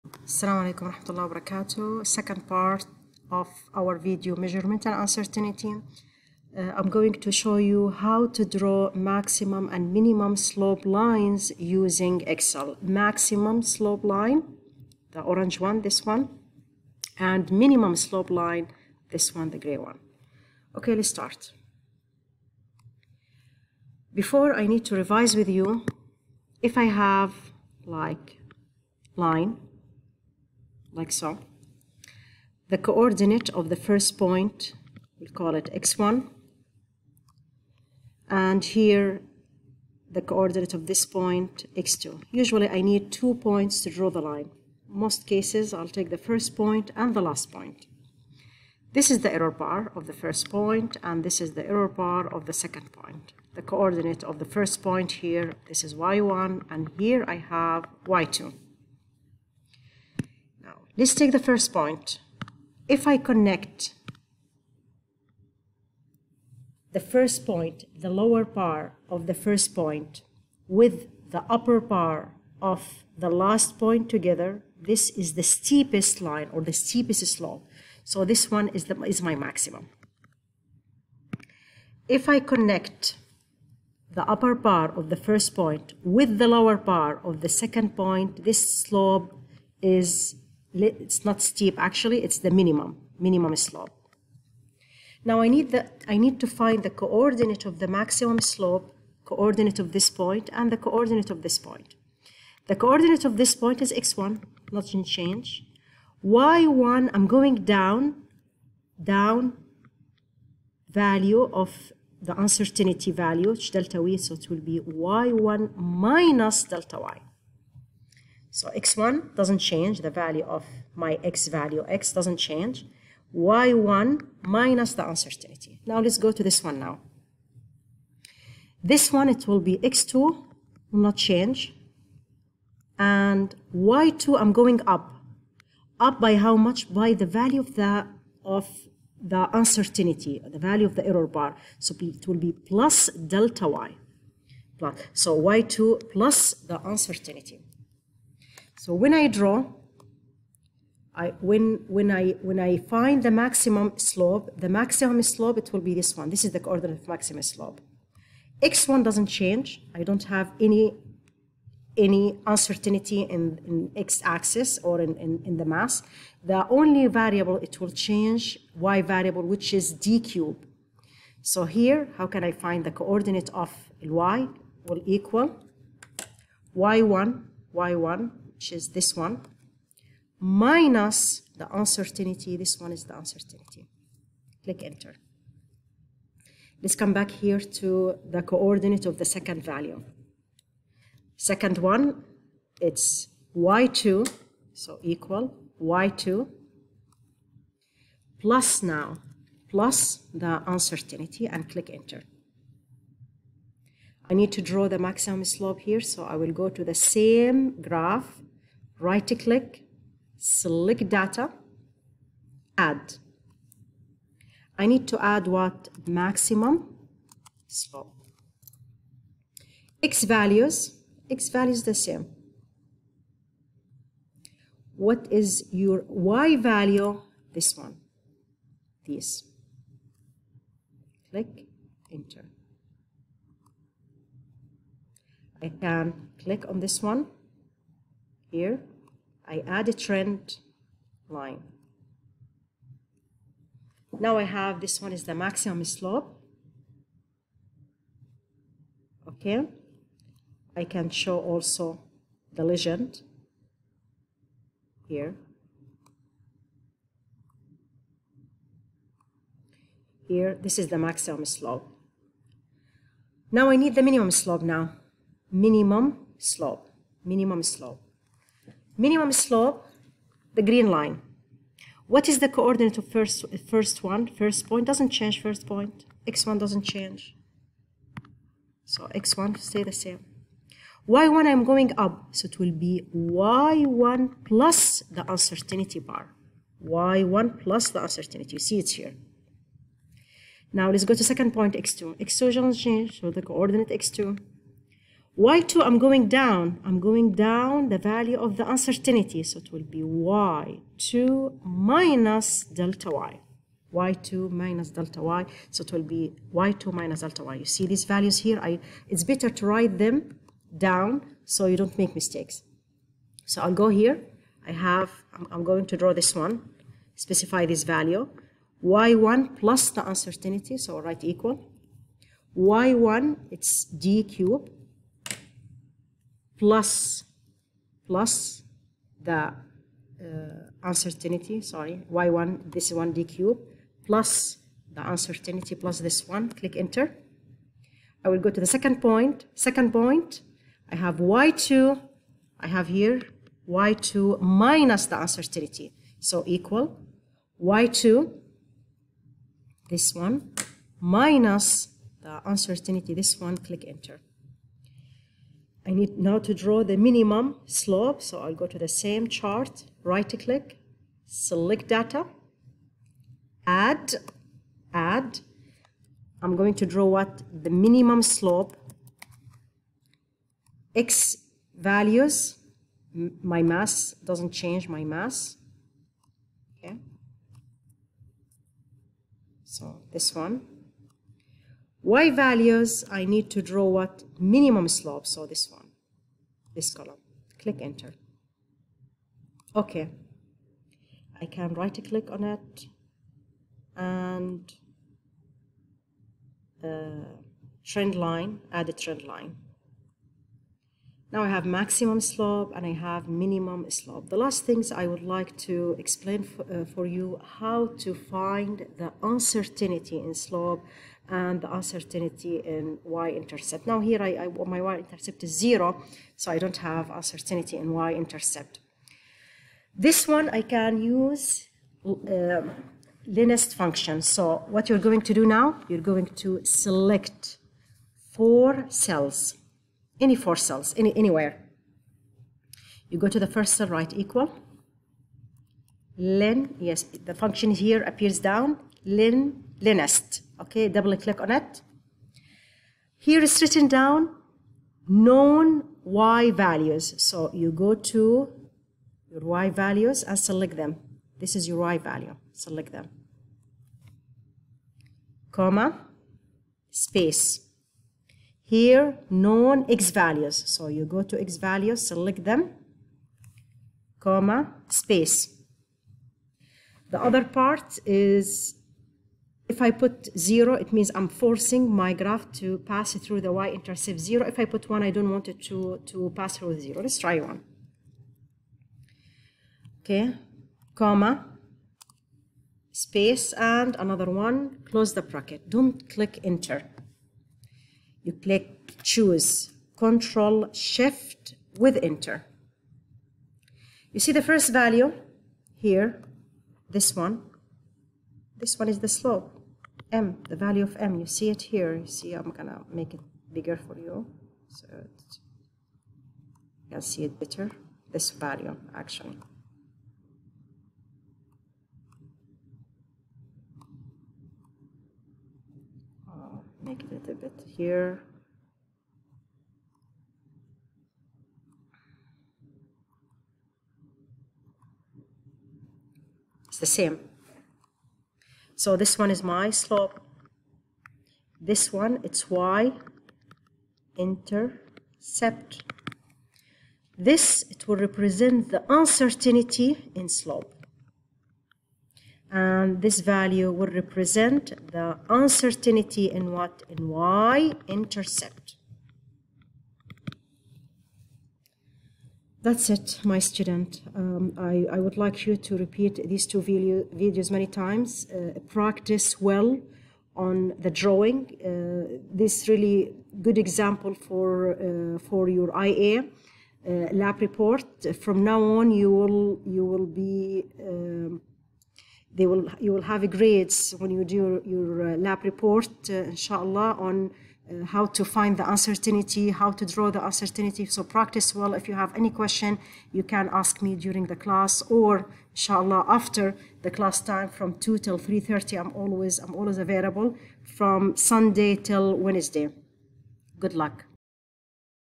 How to draw Maximum and Minimum Gradients(Slopes) using Excel -P 2
IB PHYSICS-
0:00 How to draw maximum slope (gradient) using excel.
4:30 How to draw the minimum slope (gradient)
6:45 Calculate the uncertainty in a gradient using LINEST function in excel.

Aims:
Calculate the uncertainty in a gradient using LINEST function in excel.
Draw the error bars using excel.
Draw maximum and minimum slope lines using excel
Part 1: